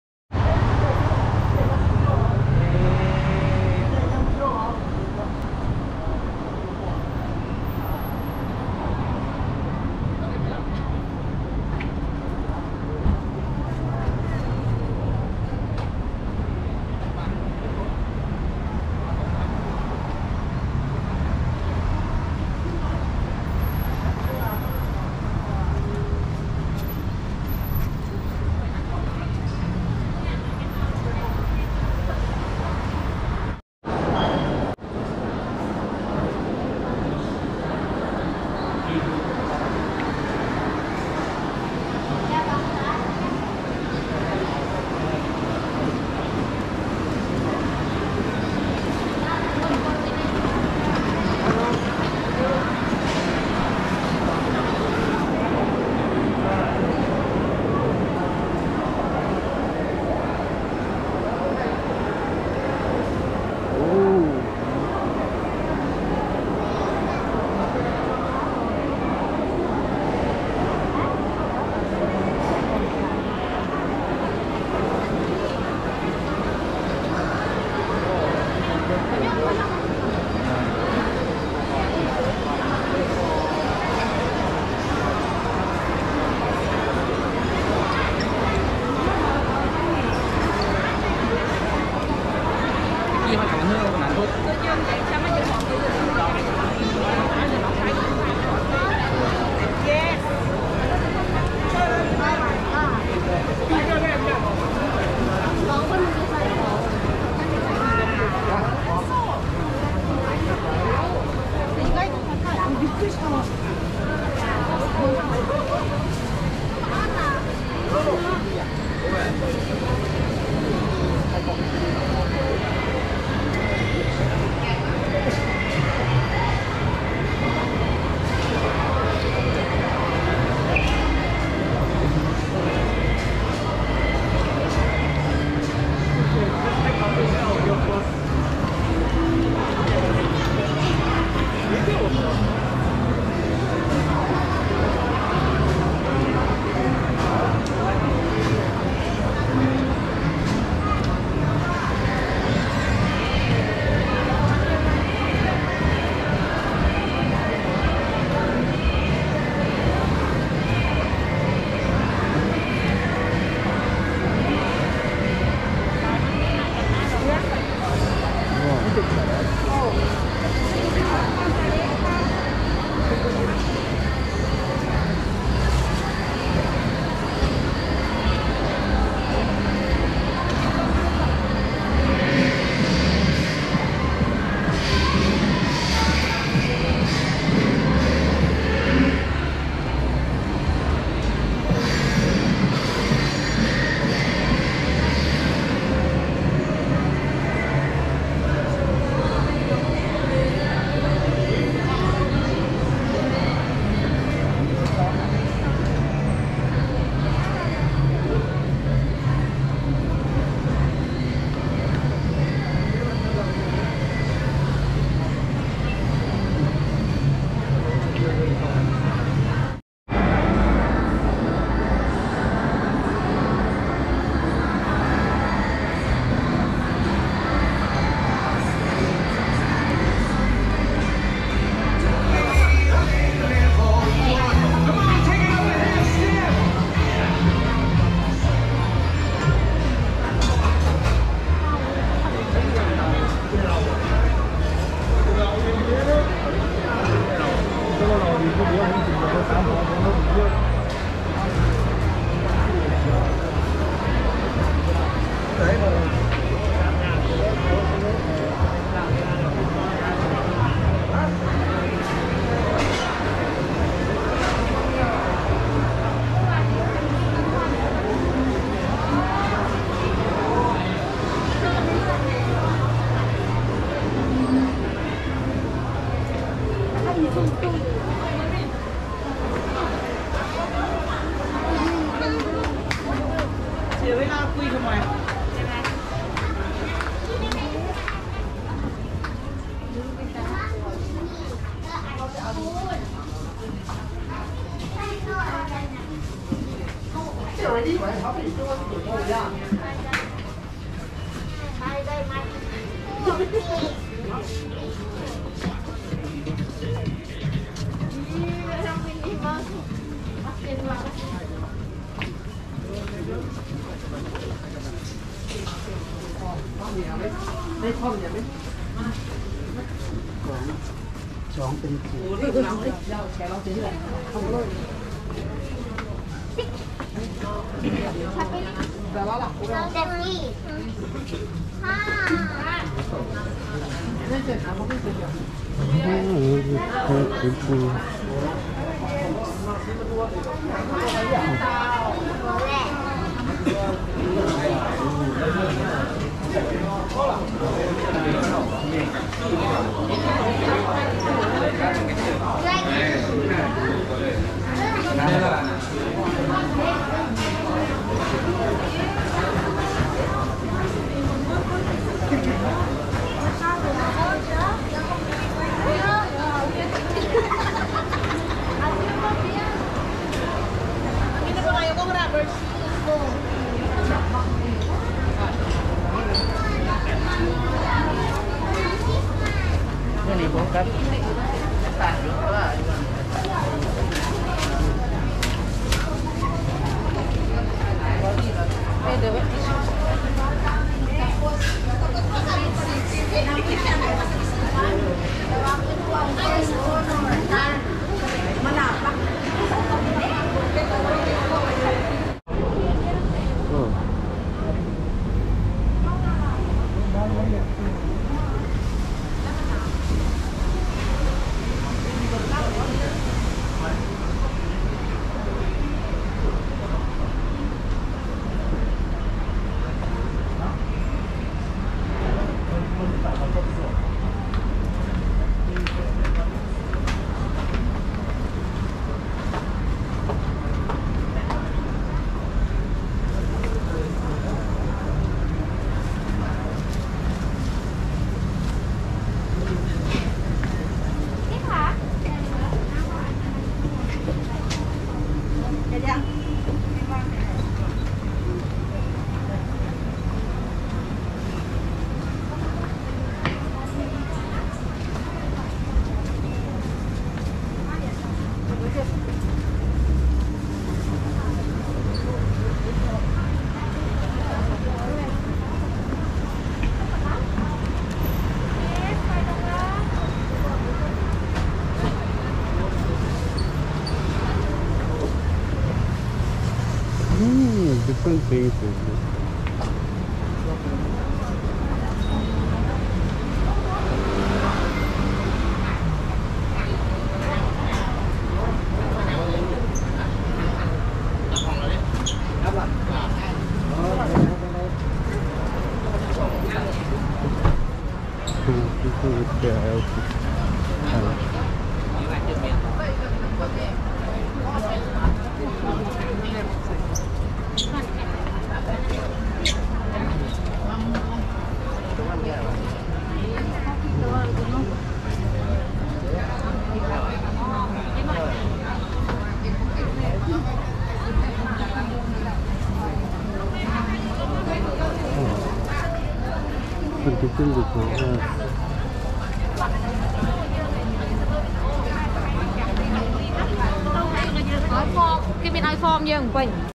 Little Thailand In Singapore 🇹🇭🇸🇬 | ลิตเติ้ลไทยในสิงคโปร์ | Golden Mile Complex
Golden Mile Complex is a high-rise commercial and residential building on Beach Road in Kallang, Singapore, within walking distance to Nicoll Highway MRT station